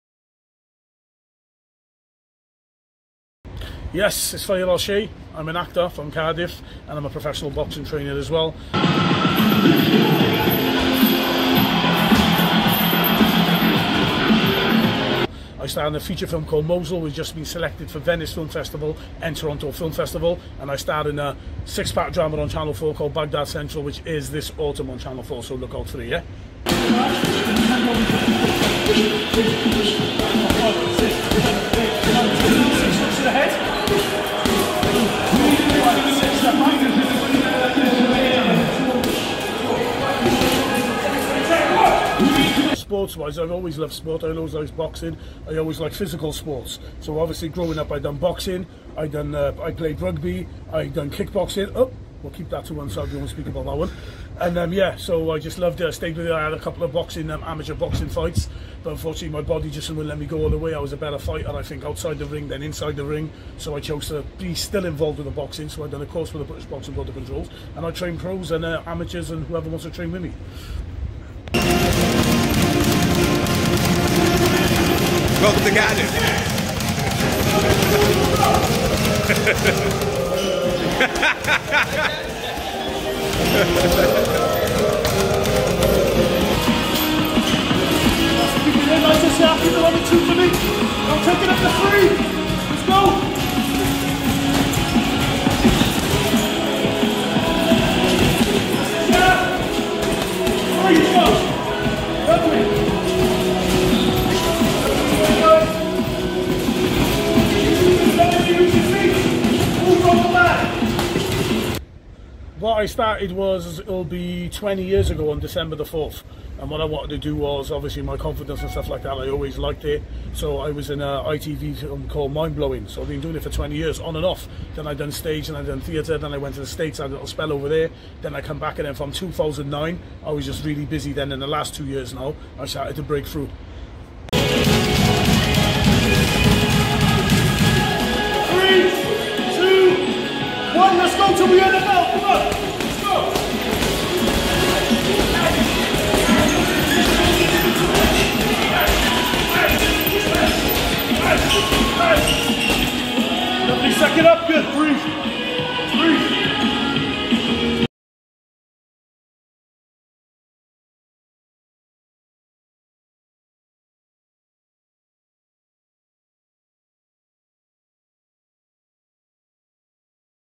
Actor/Boxer Thaer Al-Shayei gives us the lowdown
A brief chat with local celebrity Thaer Al-Shayei.